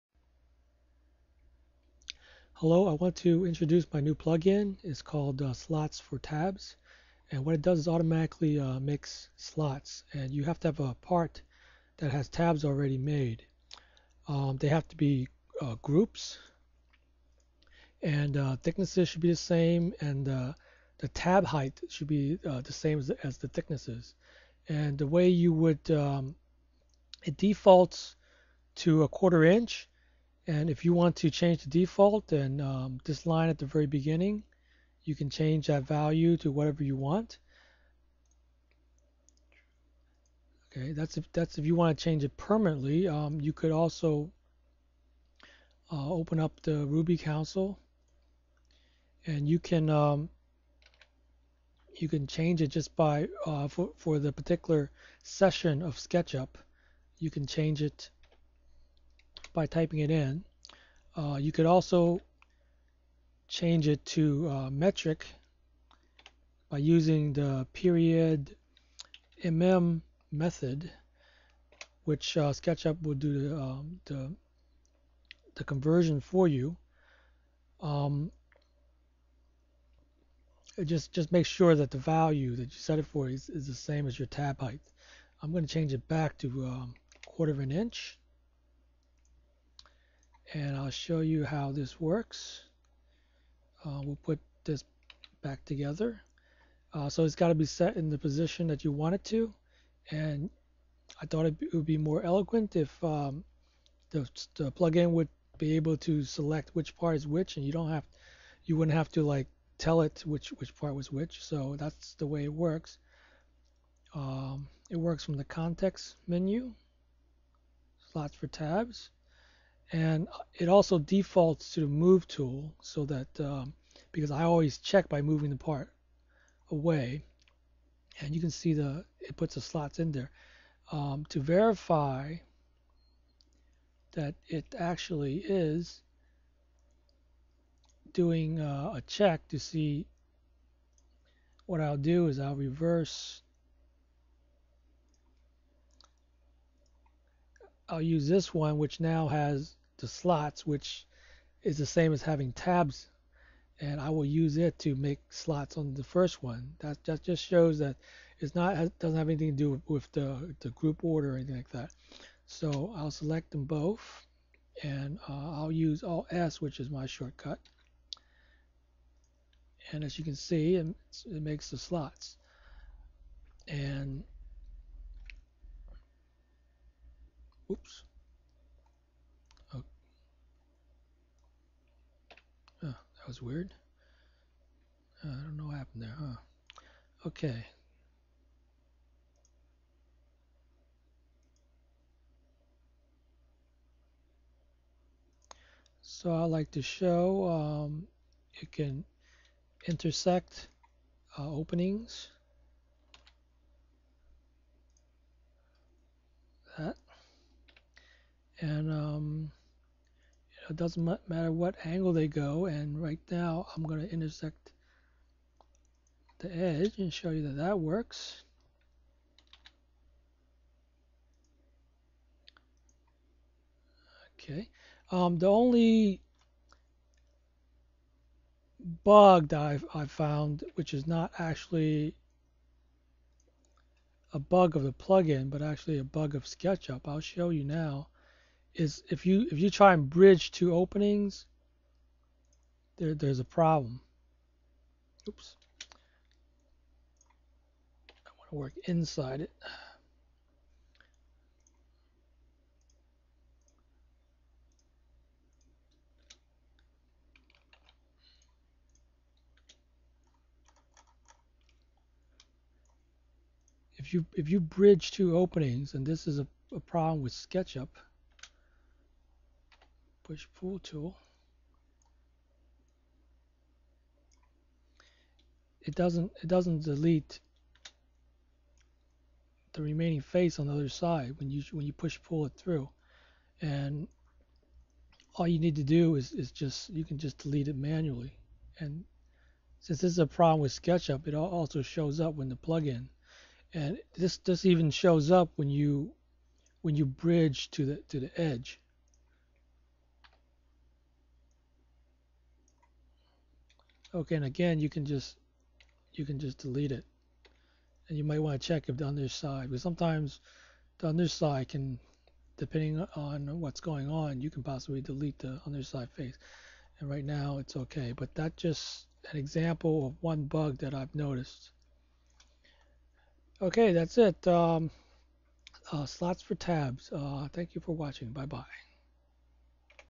Slots 4 Tabs - sketchup plugin
This is my new plugins, that automatically makes slots.  Get it from either of these links: